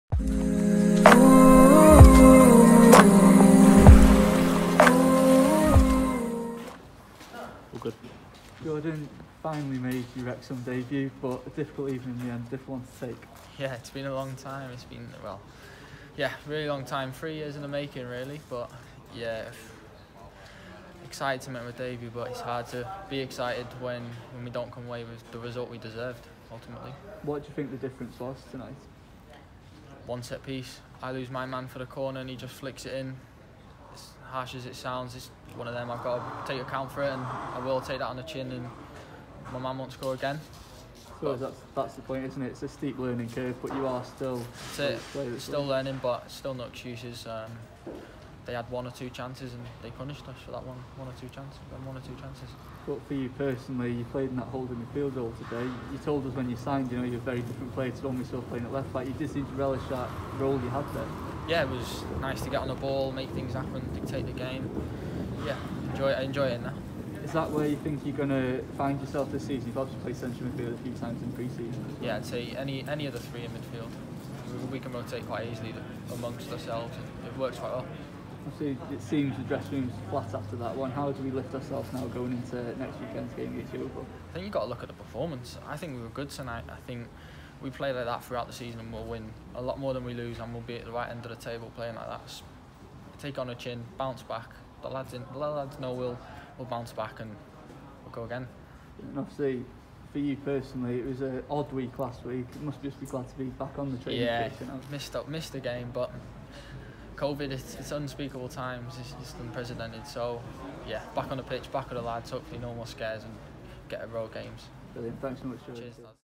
INTERVIEW | Jordan Davies on his Wrexham debut
Jordan Davies chats after finally making his Wrexham debut, as the Reds lose out at Solihull.